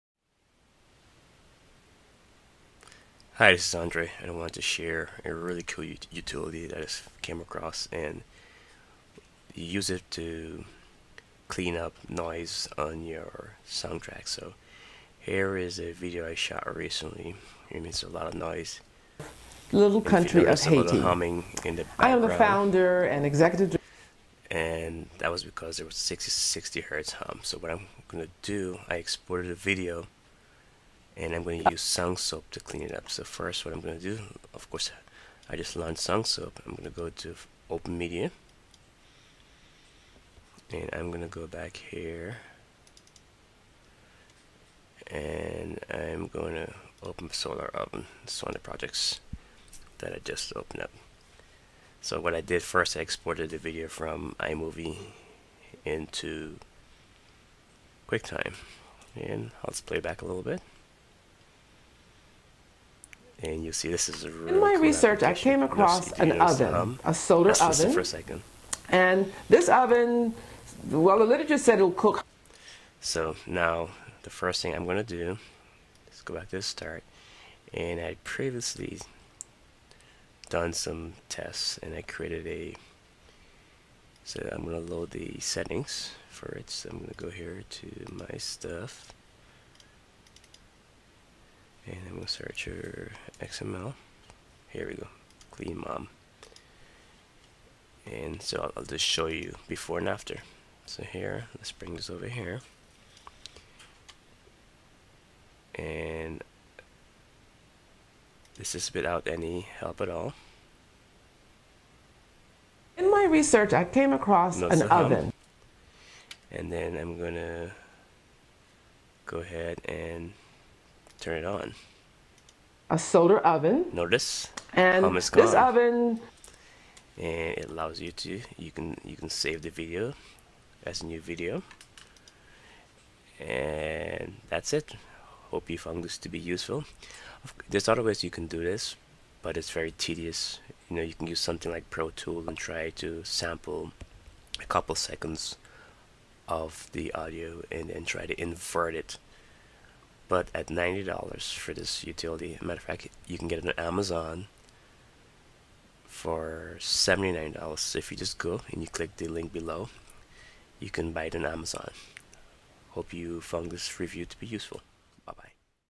Bias SoundSoap Review audio and noise reduction software
Watch as I use Bias SoundSoap to clean up the a video clip.  I remove hums and AC noise noise in less than two minutes.  This is a really cool piece of noise reduction.  You will appreciate this software if you have ever tried using phase cancellation in Pro Tools to get rid of background noise.  After spending countless hours in ProTools trying to figure this out, Bias SoundSoap is a excellent audio noise reduction software.  It is well worth the small price.